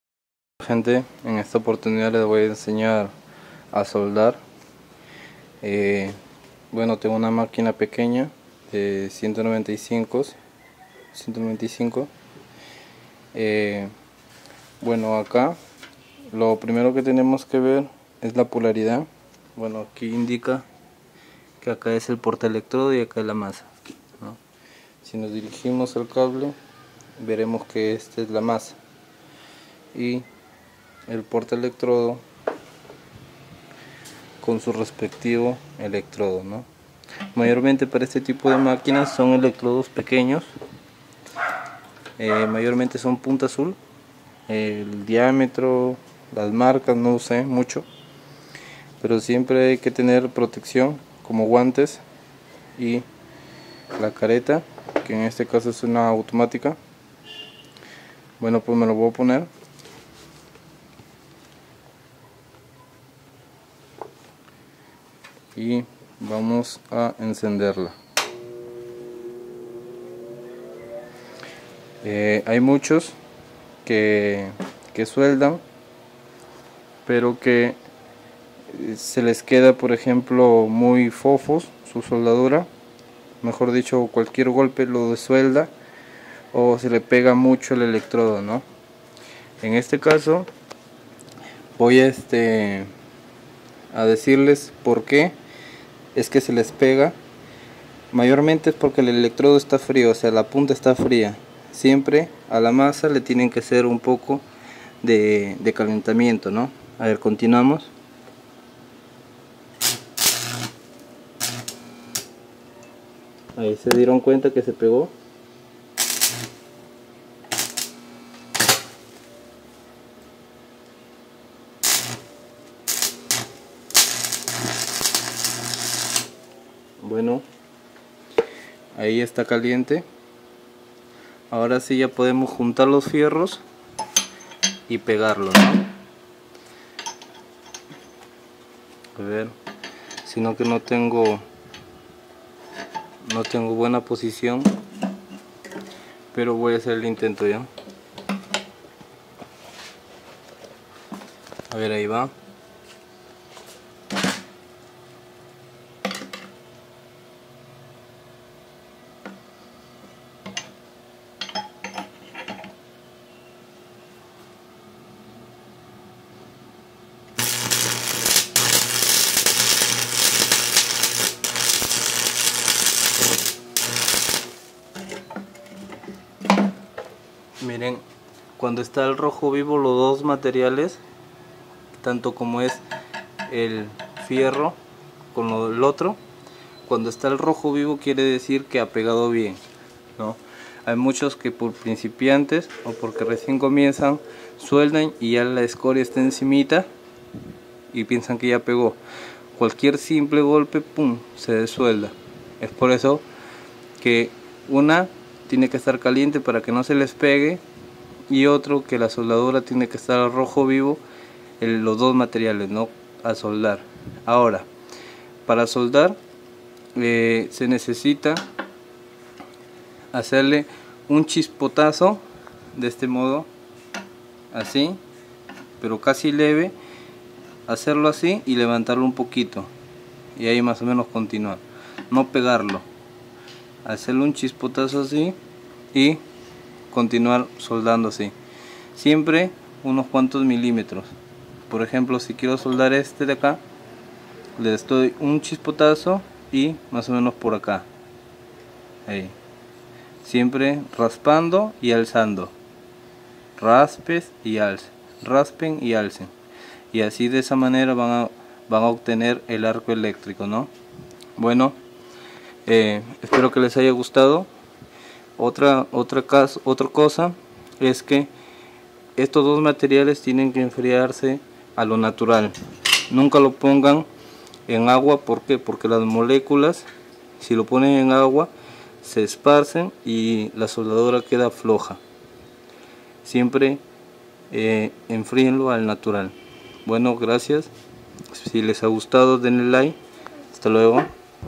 como soldar con electrodos - truco para que no se pegue y no quede debil
espero le alla gustado. este video es dirigido mayormente para la gente q recien comienza. gracias

como aprender a soldar
como soldar con electrodo punta azul